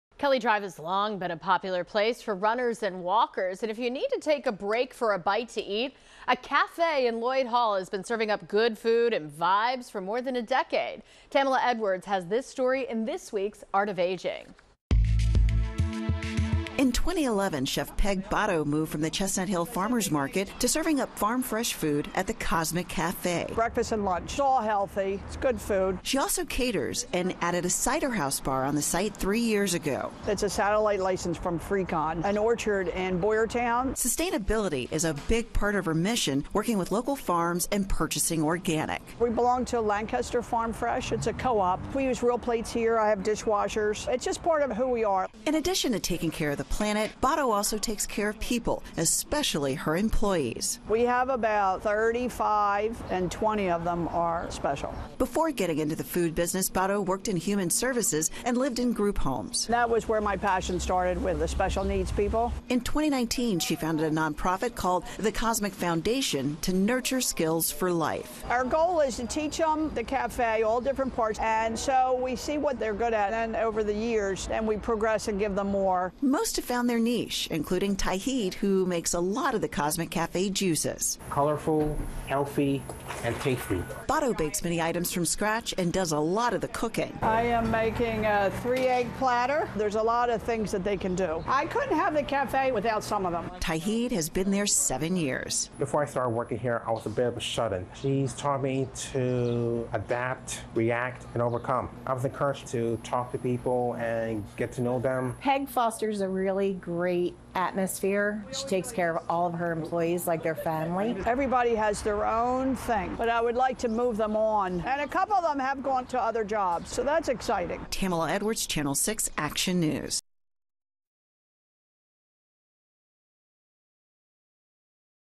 Cafe serving up scratch-made food on Philly's historic Boathouse Row
Chef Peg Botto from Cosmic Cafe has been serving up good food along with good vibes for over a decade at Lloyd Hall on Philadelphia's historic Boathouse Row.

The cafe, which opened in 2011, serves breakfast and lunch and is committed to serving up healthy, "good food."